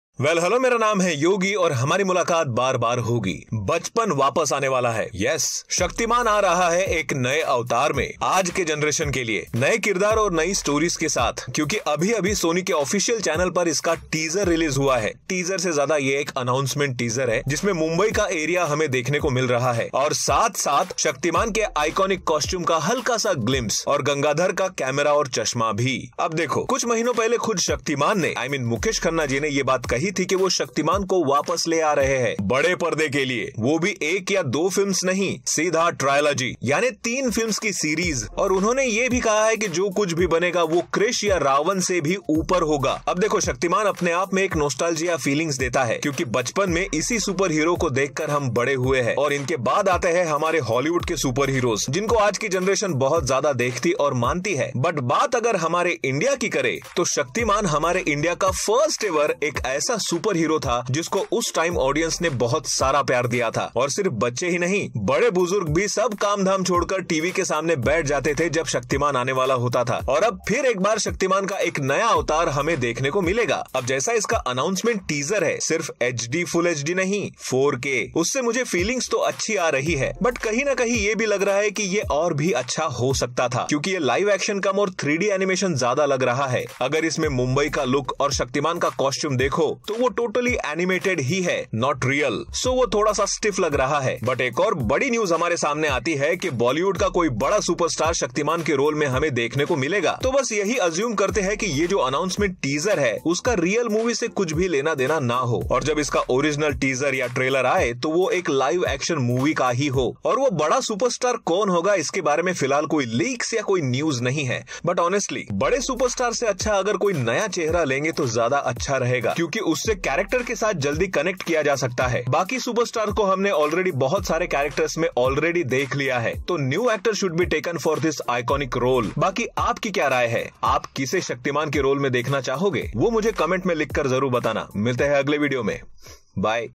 Shaktimaan Movie TEASER REVIEW | Yogi Bolta Hai
Shaktimaan Teaser Review
Shaktimaan Trailer Review
Shaktimaan Announcement Review
Shatimaan Teaser Review By Yogi Bolta Hai

After the super success of our many superhero films in India and Globally, its time for our desi Superhero!
Sony Pictures International Productions is set to bring ‘Shaktimaan’ to the big screen, and will recreate the magic of the iconic superhero, to be headlined by one of India’s superstars.
Will join hands with Brewing Thoughts Private Limited, and Mukesh Khanna's Bheeshm International.